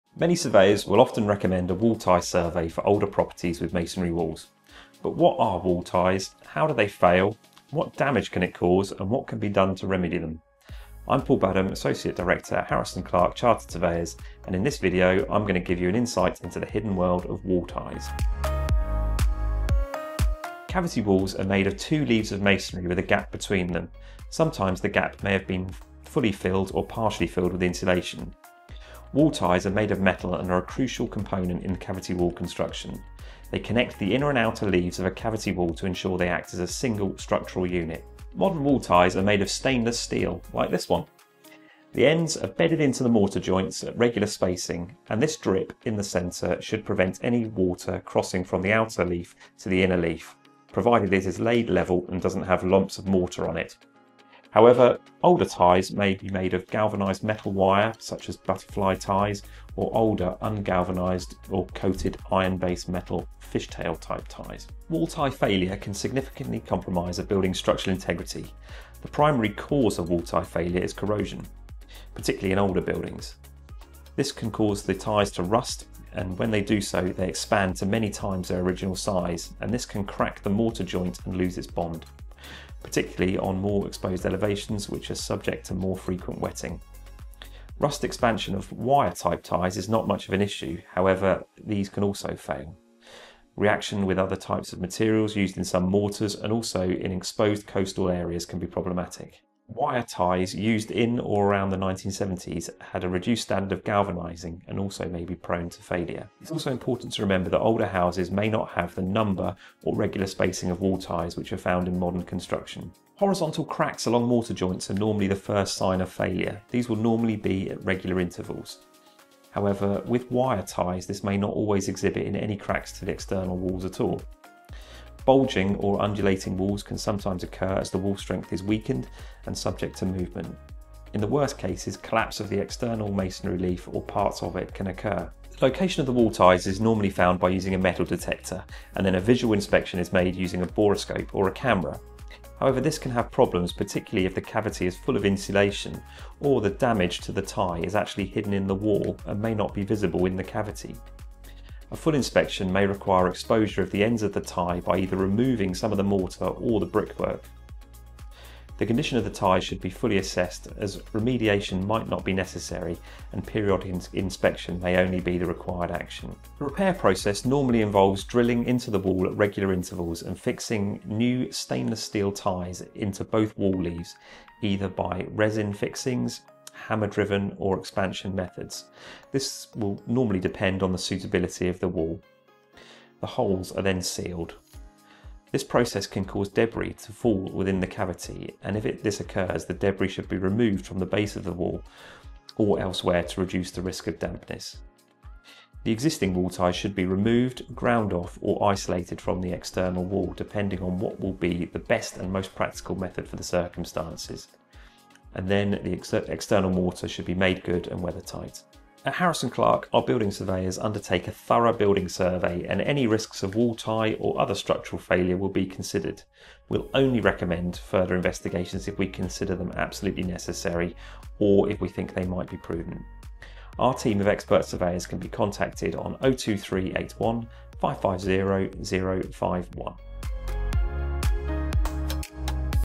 Wall Tie Failure EXPLAINED
Timestamps
0:00 Introduction
0:22 What Are Wall Ties?
1:54 What Are The Signs of Wall Tie Failure?
2:17 How Can You Inspect Wall Ties?
2:48 Can Wall Ties Be Repaired?
3:30 How Can Harrison Clarke Help?

Are you seeing cracks in your walls? Bulging brickwork?
You might be dealing with wall tie failure - a common and serious issue in older masonry cavity walls.

In this video, we break down:
What wall ties actually do
Why they fail (especially in pre-1980s homes)
The warning signs to look for
How surveyors detect failed ties
What repairs involve (and when they’re needed)
Don't ignore these signs - failing wall ties can lead to collapsed walls.

Whether you're a homeowner, buyer, or property investor, knowing how to spot wall tie issues can save you from major repair costs later.